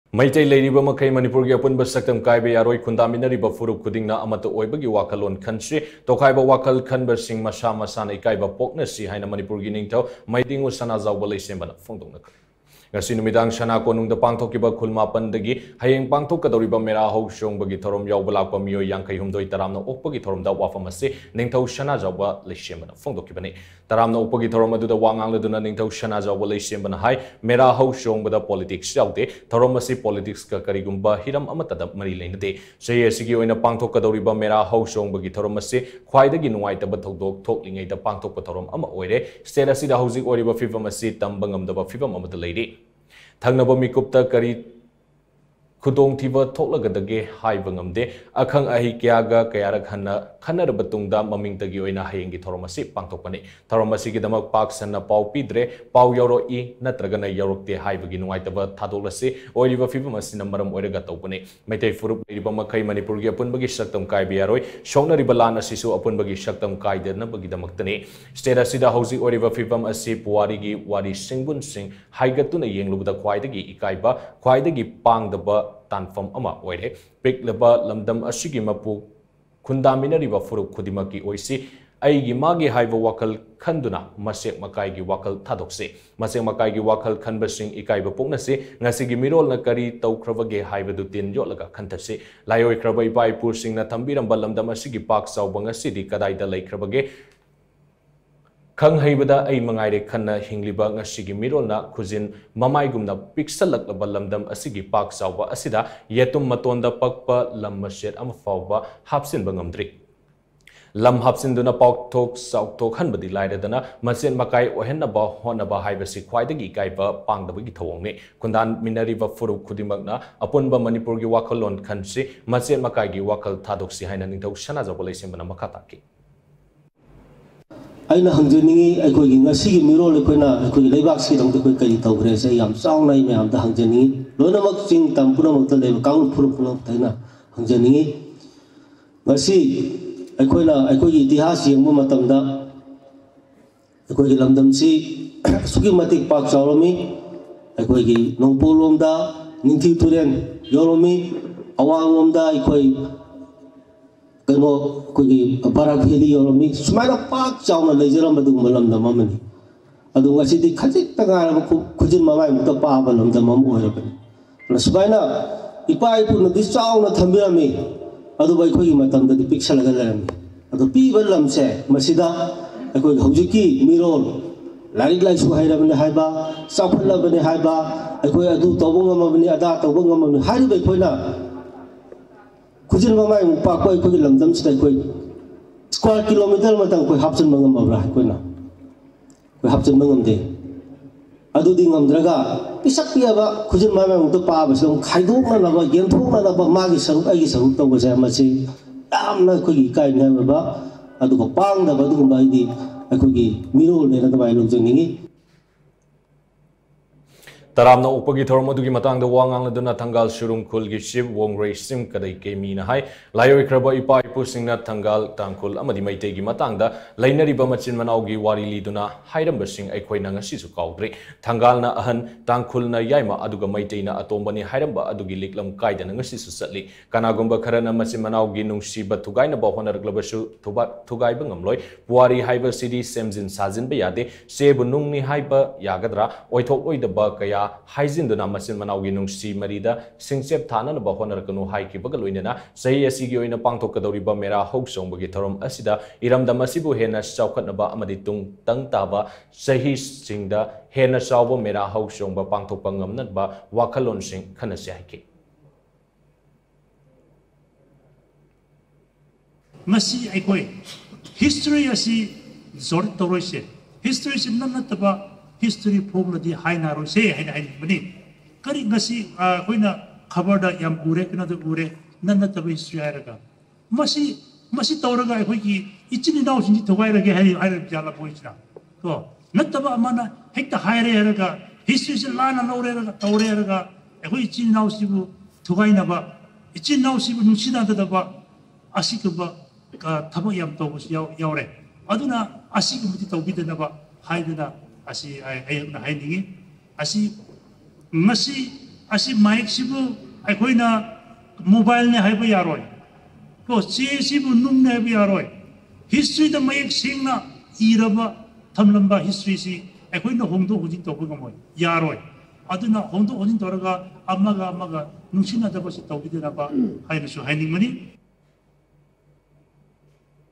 INTEGRITY OF MANIPUR CANNOT BE COMPROMISED UNTILL MEITEI EXIST: LEISHEMBA SANAJAOBA
Meitei leiriba makhei Manipur gi apunba shaktam kairoi; khundaminnariba phurup khuding makna amatta oibagi wakhal khanshi haina Manipur gi ningthou Meidingoo Sanajaoba Leishemba na phongdorakli.